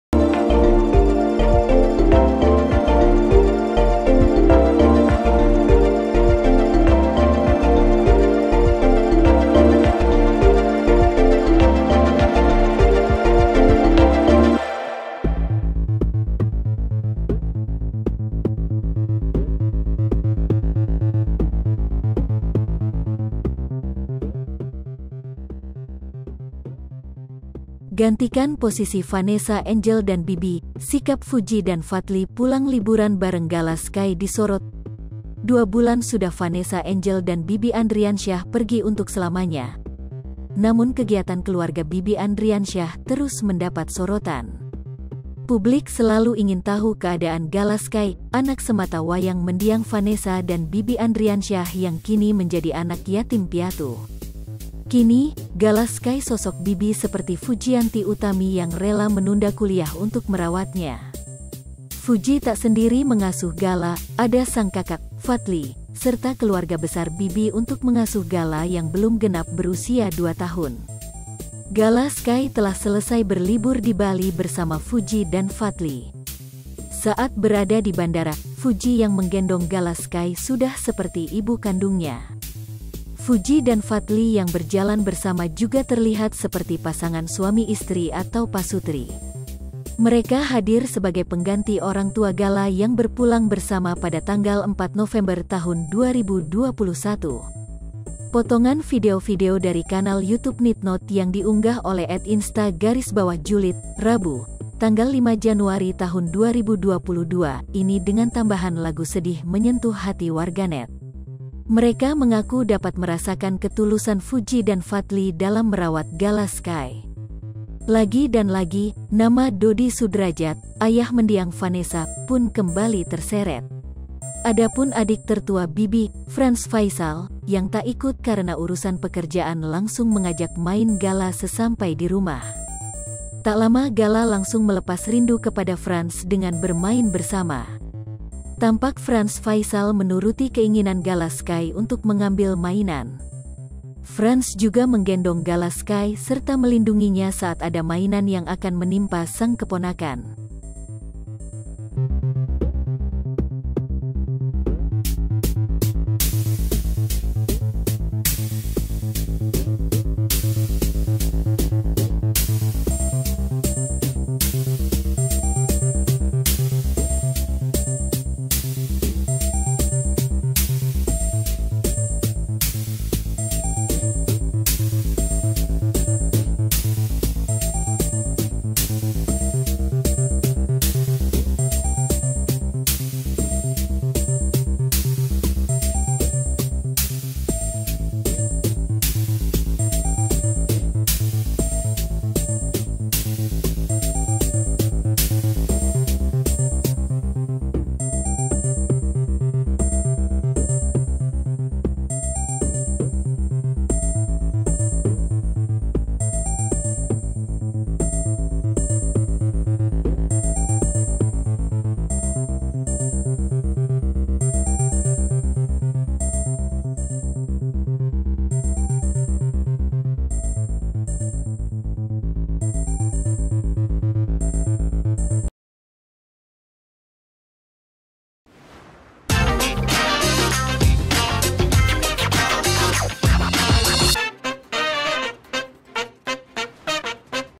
Gantikan Posisi Vanessa Angel dan Bibi, Sikap Fuji dan Fadly Pulang Liburan Bareng Gala Sky Disorot
Publik selalu ingin tahu keadaan Gala Sky, anak semata wayang mendiang Vanessa dan Bibi Andriansyah yang kini menjadi anak yatim-piatu.

Kini, Gala Sky sosok bibi seperti Fujianti Utami yang rela menunda kuliah untuk merawatnya.

Fuji tak sendiri mengasuh Gala, ada sang kakak, Fadly, serta keluarga besar Bibi untuk mengasuh Gala yang belum genap berusia 2 tahun.

Gala Sky telah selesai berlibur di Bali bersama Fuji dan Fadly.

Saat berada di bandara, Fuji yang menggendong Gala Sky sudah seperti ibu kandungnya.

Fuji dan Fadly yang berjalan bersama juga terlihat seperti pasangan suami istri atau pasutri.

Lihat informasi terkini bersama kami, di:

Palembang.tribunnews.com